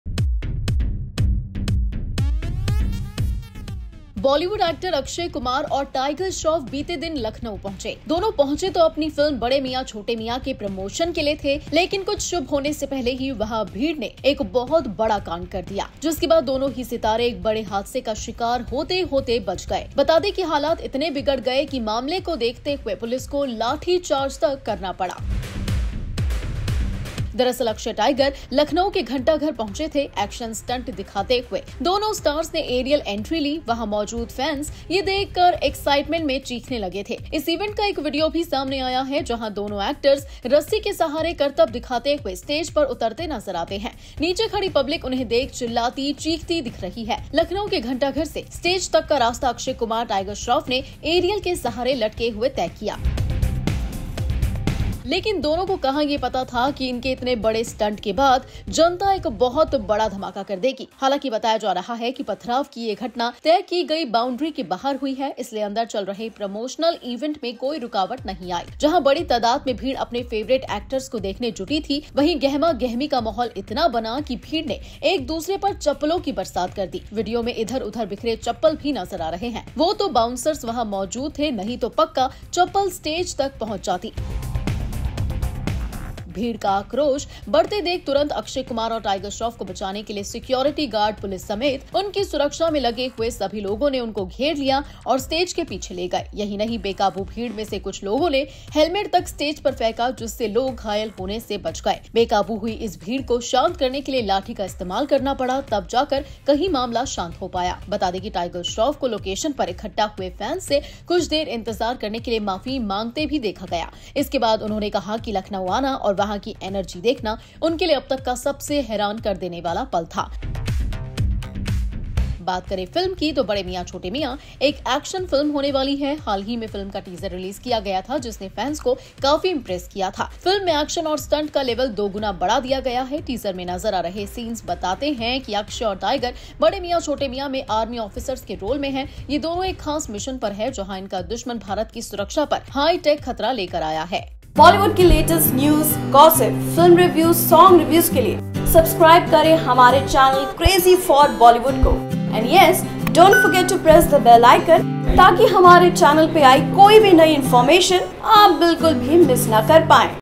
Akshay Kumar & Tiger Shroff's 'BMCM' promotional event in Lucknow turns chaotic, crowd hurl slippers
Bade Miyan Chote Miyan: Akshay Kumar-Tiger Shroff's trip to Lucknow marred with ruckus, here's why.